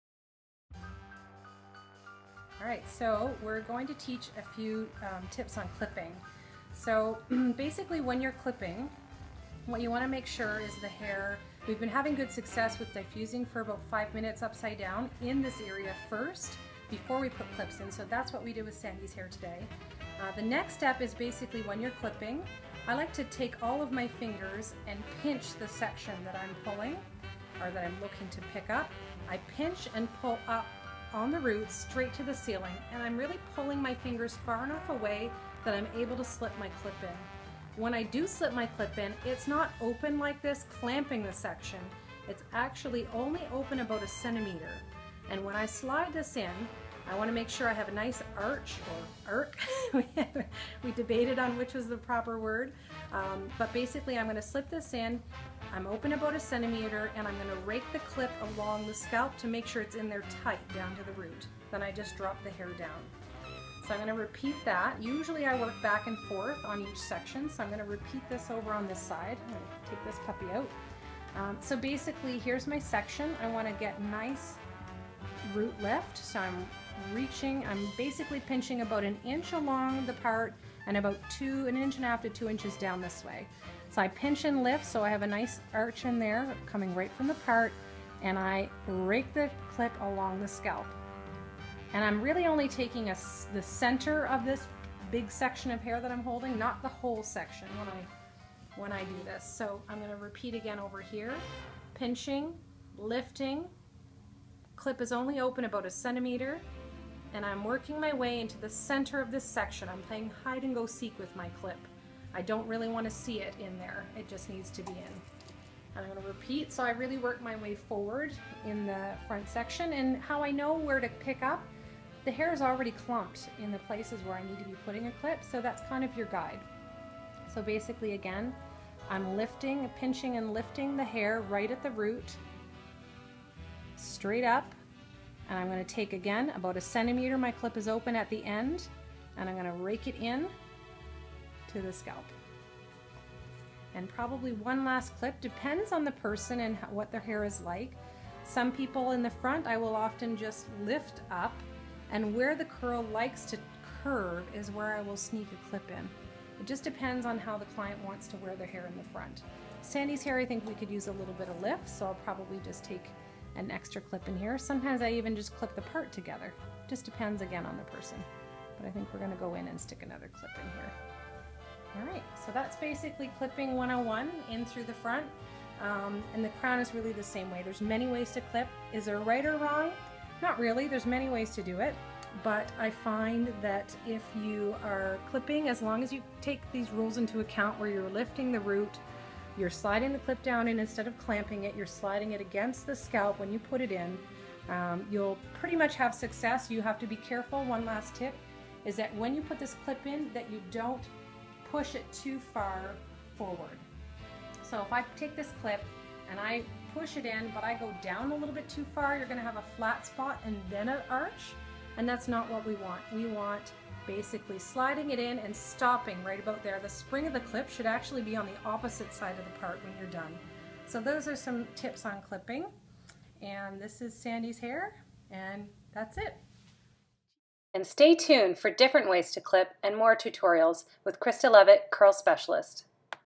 How To Clip Curly Hair: Clipping Hair for Volume
A short tutorial from Krista Leavitt~Curl Specialist, ranked one of Canada's Top Curl Specialists; on how to clip your roots for volume. Process is done on damp hair with double prong pin curl clips. (FYI-Make sure there is no bar joining the two prongs together).  A How-To for naturally curly haired girls to create more volume in the root area for when curls are dry and finished.

It's not professionally done by any means but it gets the job done.

Love,

Krista Leavitt xxx